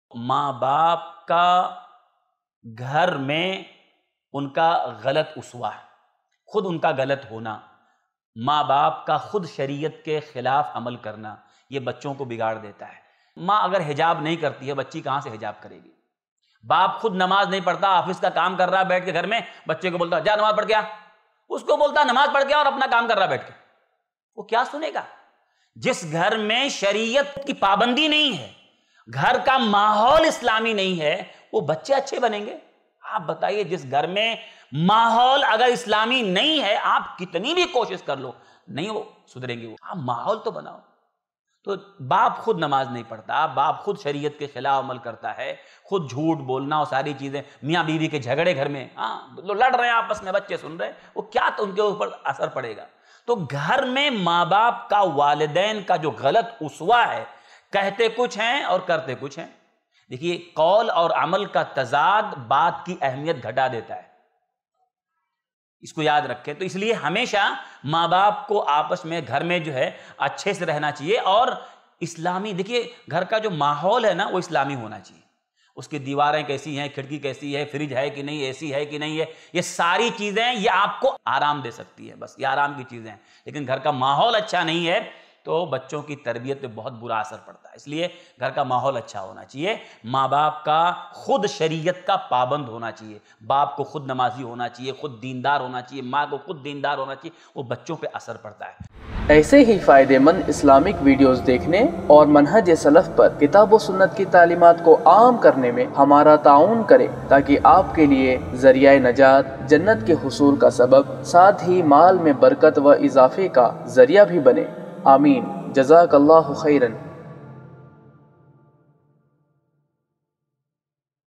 Agar Aap Chahte Hai Ki Aapki Aulaad Deen Par Amal Kare Toh Sabse Pahle Ye Kare I Fakhruddin Salafi
Bank Account Details:
ILM FOUNDATION
ICICI Bank
Savings account
IFSC CODE-ICIC0001028